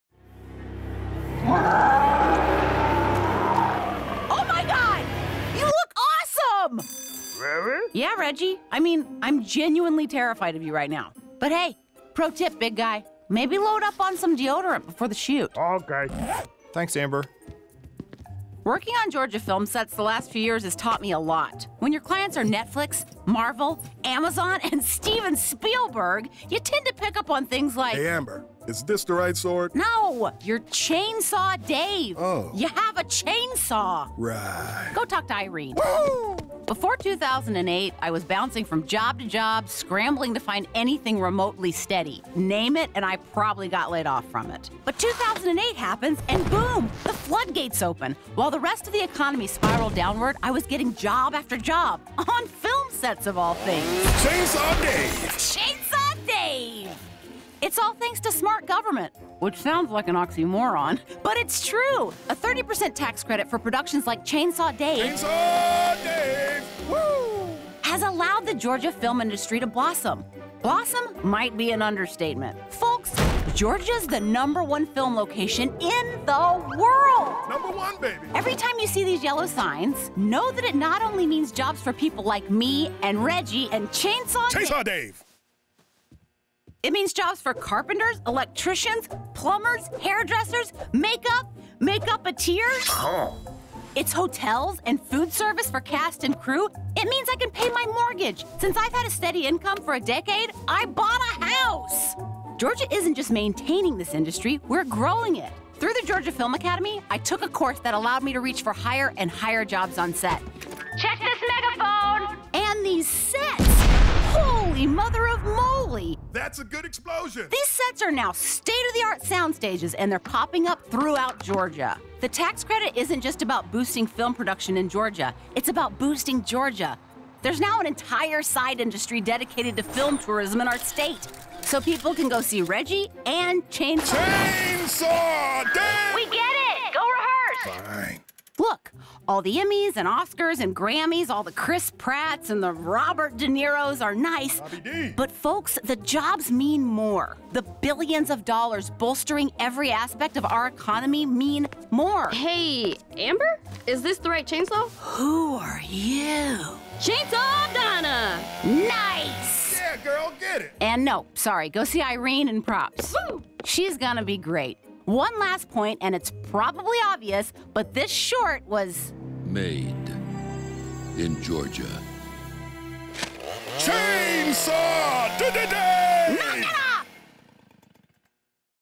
Film Works
The Georgia Department of Economic Development partnered with Floyd County Productions to produce an animated short detailing the explosive growth of Georgia’s film industry. This whimsical narrative is led by the animated version of Amber Nash who is an American actress and Georgia native who got her start at Dad’s Garage Theatre in Atlanta. She takes viewers through the many facets of the film industry, including workforce development and the types of jobs generated, studio development, as well as the tourism industry that has sprung up across the state of Georgia where famous movies and T.V. shows have been filmed. A complementary web experience is located at Georgia.org/FilmWorks.